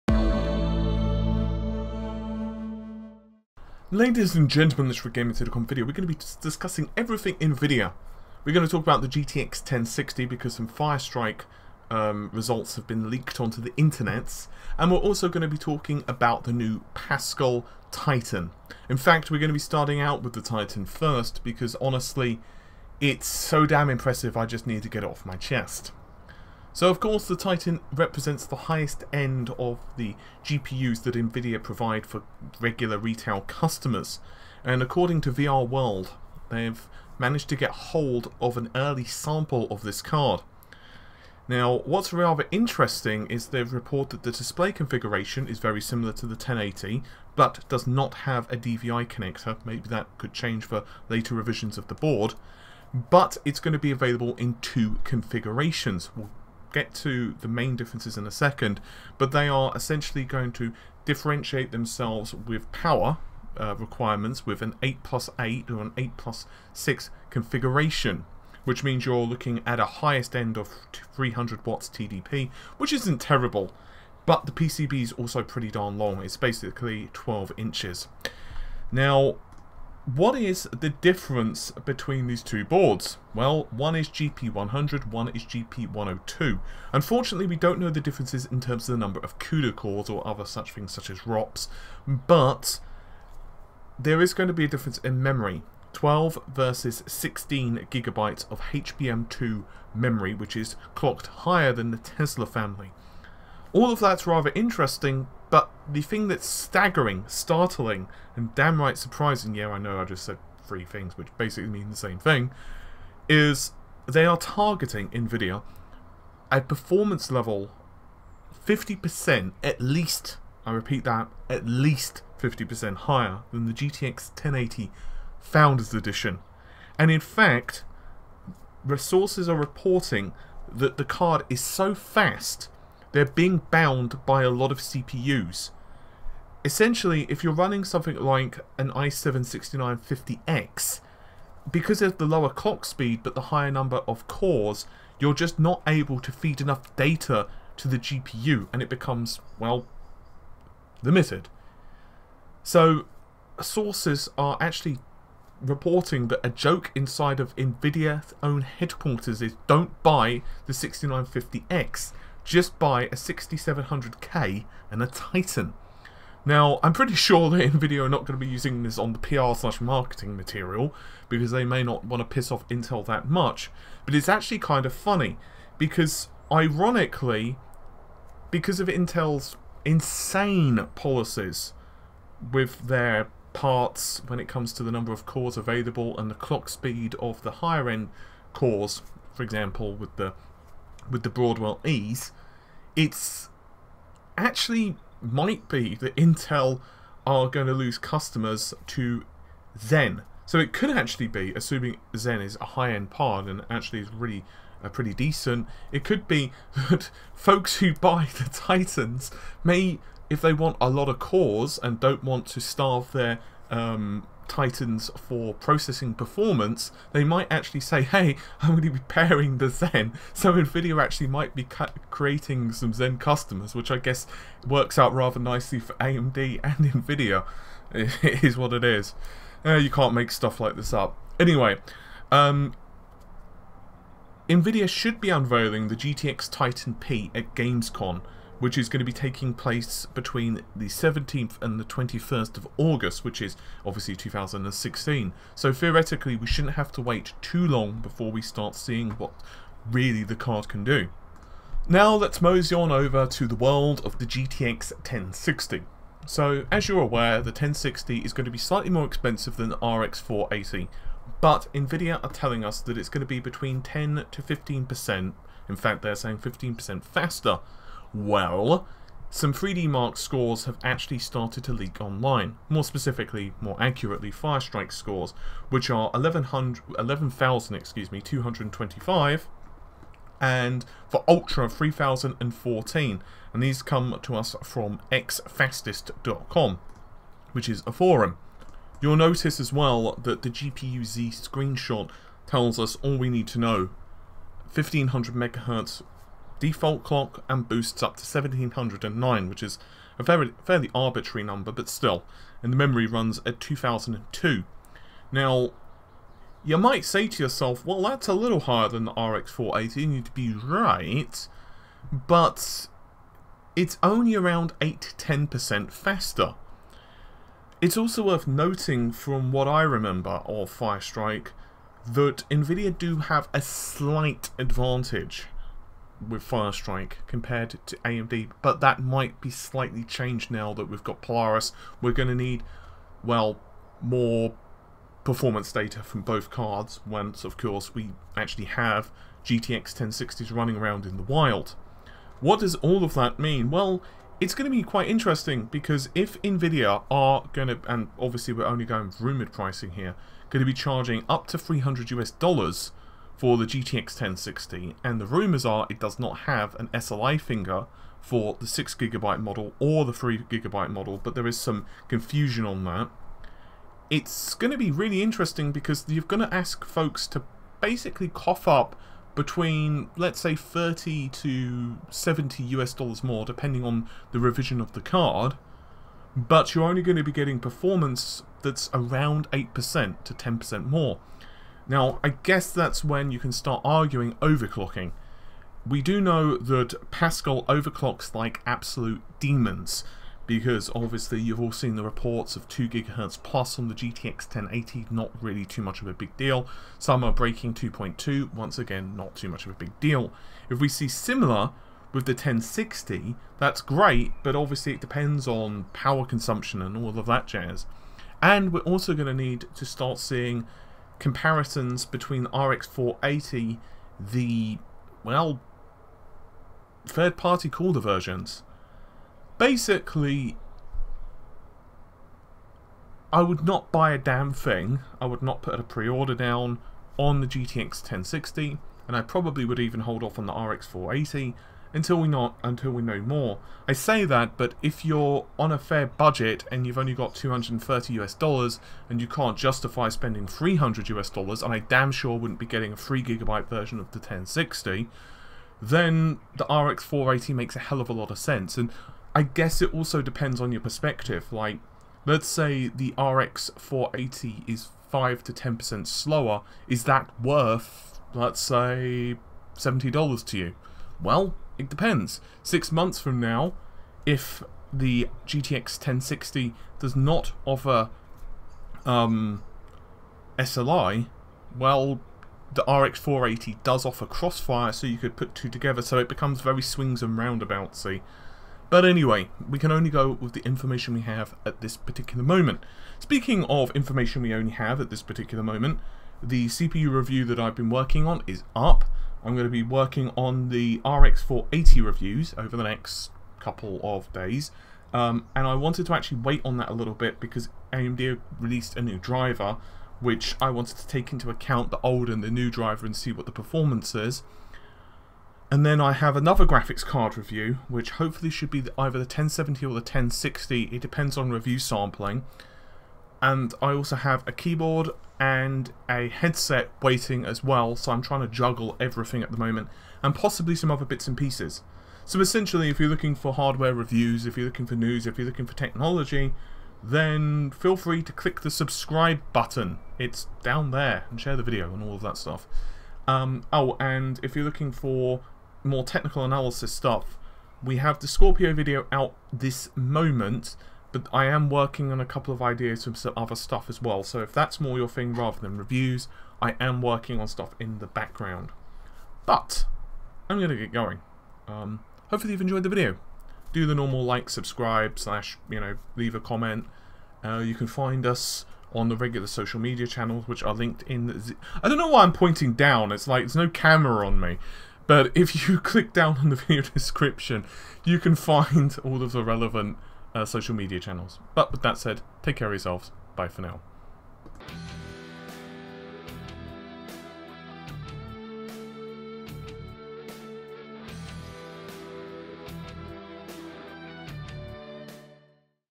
LEAK: Pascal Titan Specs & 50% Faster Than GTX 1080 | GTX 1060 FireStrike Benchmarks Leak
Nvidia's new titan based on the pascal architecture has supposedly found its way into the hands of a website known as VR world, and they're reporting that the pascal titan will be at least 50 percent faster than the GTX 1080 founders edition card.

The GP100 and GP102 based titans will have 16 or 12GB HBM2 memory and the graphics card will also come with either 2 8 pin power connectors or an 8 and 6 pin power connector.

The Titan will be announced at gamescon 2016. The 1000 Titan Pascal is also said to be CPU bound, unless you have massively higher CPU clock speeds.

Furthermore, the GTX 1060 firestrike benchmarks have leaked out onto the internet, and put it about 10 percent faster than the RX 480 gpus.

The GTX 1060 is said to be released mid july, 2016.